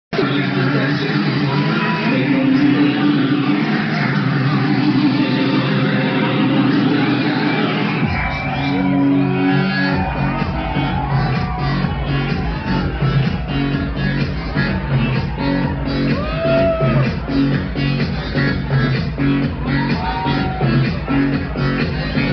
Bad Boy Bill @ Exhibition Closing 14/04/2007
The final Event of Bochums famoust club "Exhibition" with Bad Boy Bill and 20th Fingers.

"Behind the DEck Tour 2007"